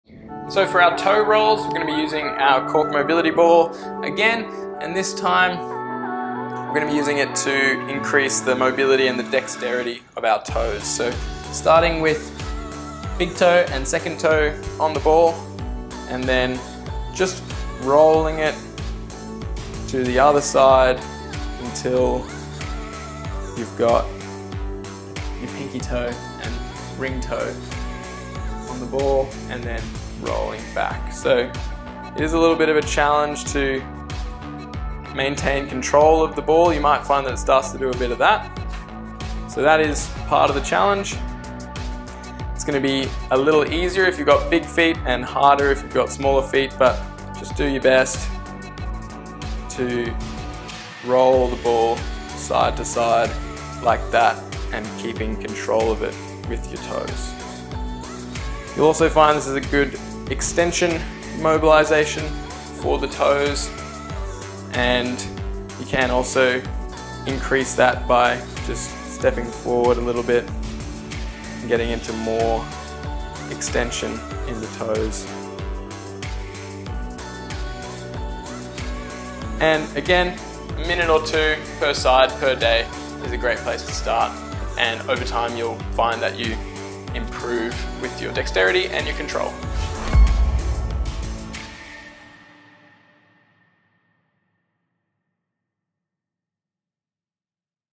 TFC Movement Snack: Toe Rolls
This video is taken from our Foot Training System which comes free with any TFC Foot Care product purchase.